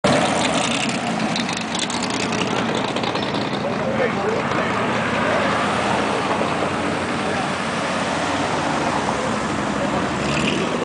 Van Nuys Boulevard 2
More clips from attending the 25th reunion of the Van Nuys Cruise night